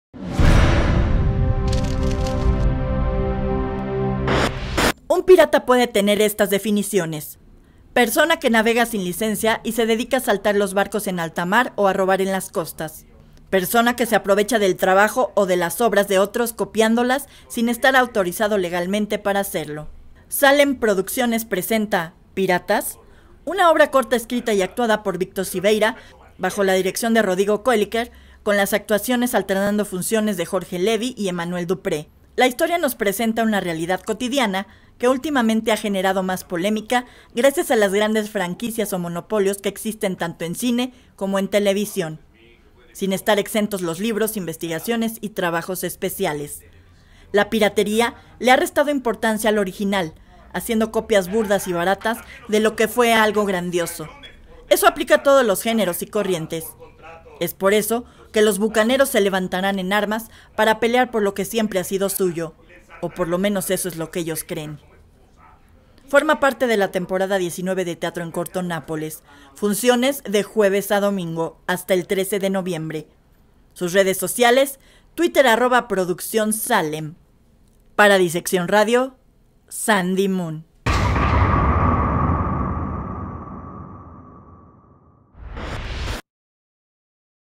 PIRATAS?
Un pirata puede tener estas definiciones: 1.- Persona que navega sin licencia y se dedica a asaltar los barcos en alta mar o a robar en las costas. 2.- Persona que se aprovecha del trabajo o de las obras de otros copiándolas sin estar autorizado legalmente para hacerlo.